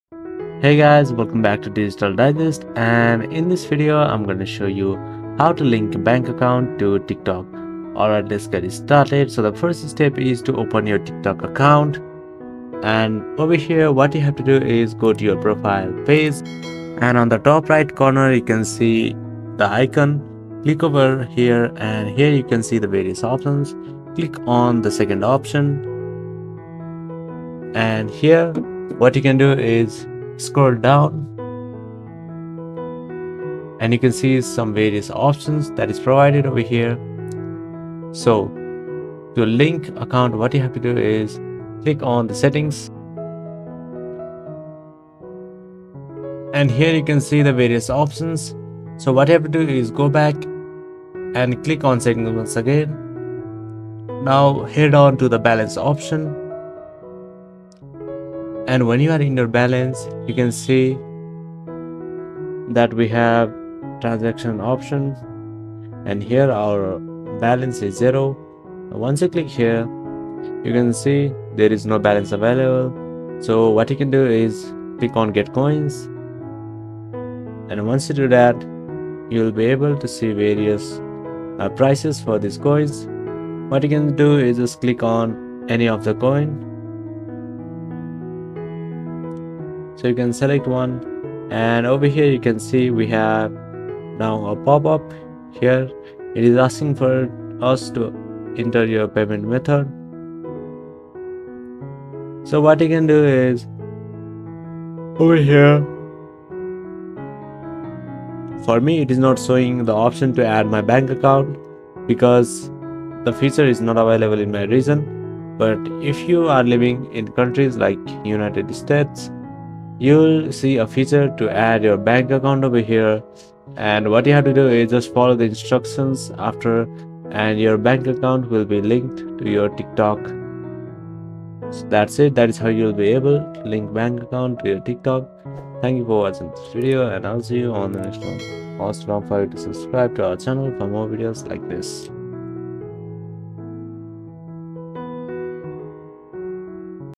How to Link Bank Account to TikTok! (Quick & Easy)
How to Link Bank Account to TikTok

Want to link your bank account to TikTok? It's quick and easy! Watch this video for a step-by-step guide on how to link your bank account to TikTok for seamless transactions.